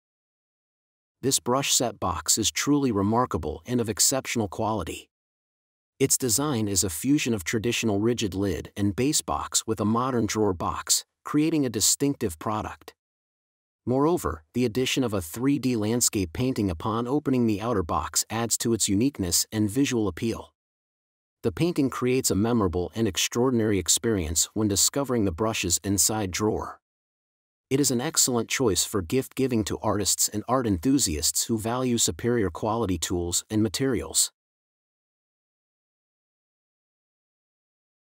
Unleash Your Creativity with Our Custom Unique Rigid Set Box for Paintbrush
Exceptional Paintbrush Set Collection Packaging Box!
This brush set box is truly remarkable and of exceptional quality. Its design is a fusion of traditional rigid lid and base box with a modern drawer box, creating a distinctive product. Moreover, the addition of a 3D landscape painting upon opening the outer box adds to its uniqueness and visual appeal. The painting creates a memorable and extraordinary experience when discovering the brushes inside drawer.

Furthermore, the box's premium feel is enhanced by the use of high-quality materials and soft touch lamination. As a result, it is an excellent choice for gift-giving to artists and art enthusiasts who value superior quality tools and materials.

Epackprinting is your one-stop solution for effortless custom packaging.